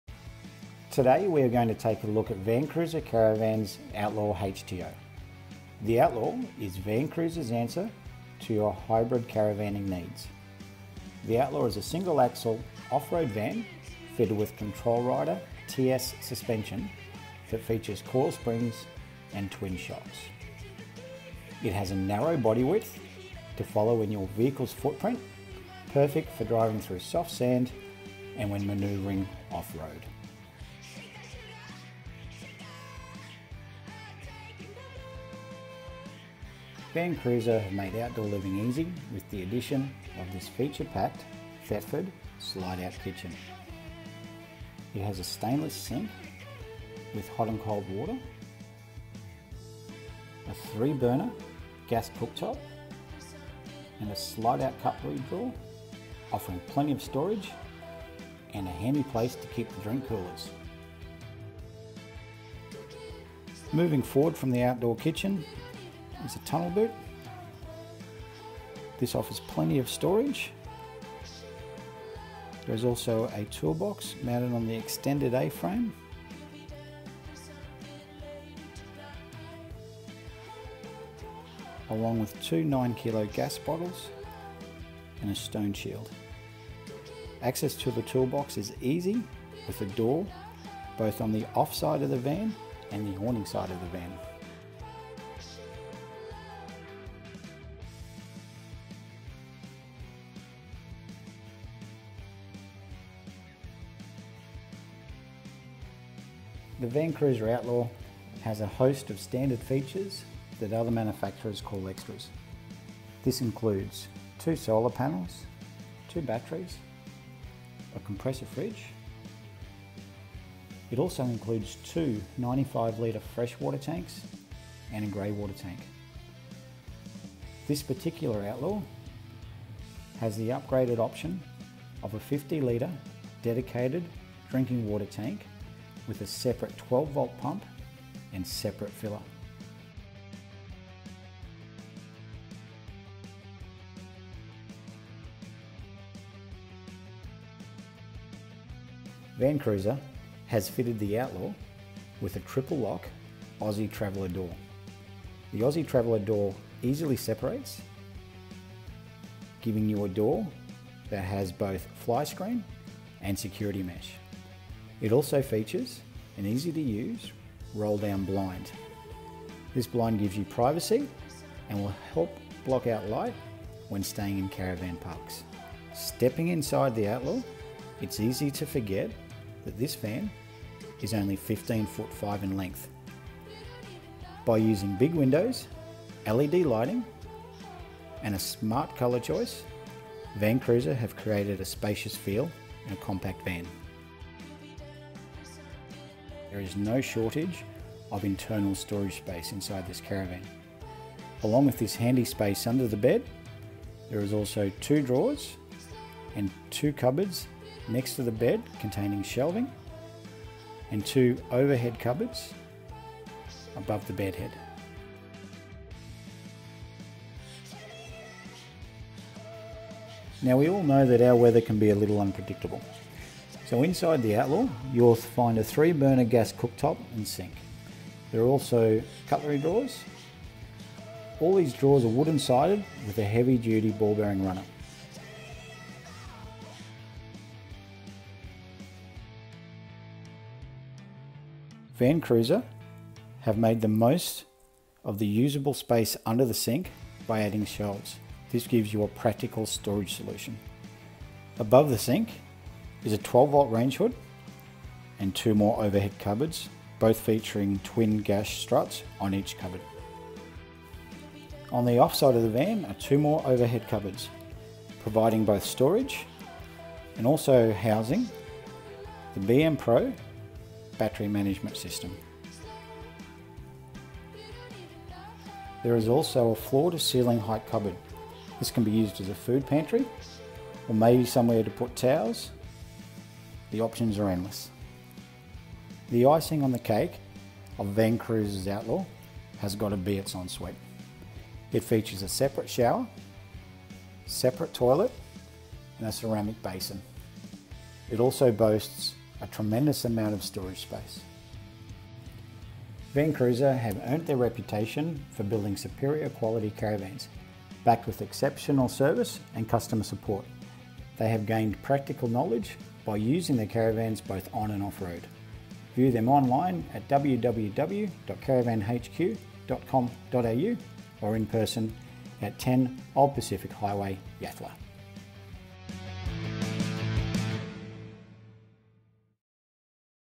2020 Van Cruiser Outlaw HTO
Van Cruiser Caravans Outlaw HTO.
At just 15’5 this is a nimble and capable off roader that is packed with features.